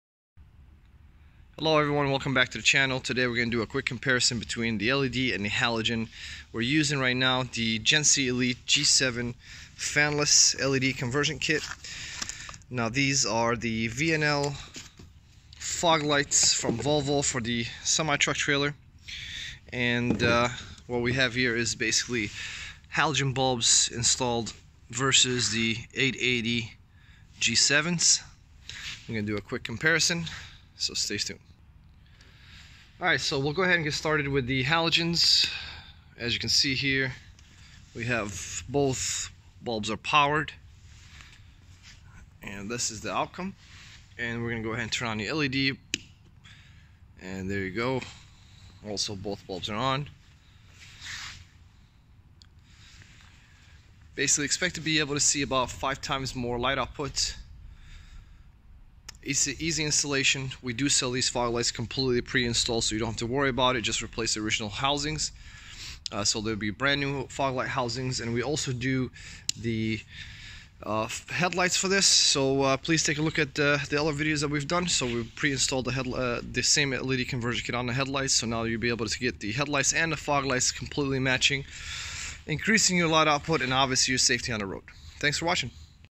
G7 Elite LED Fog Light Bulb Upgrade for Volvo VNL Trucks
Easy to do LED upgrade for VNL Semi-Trucks
Bulb Type: 880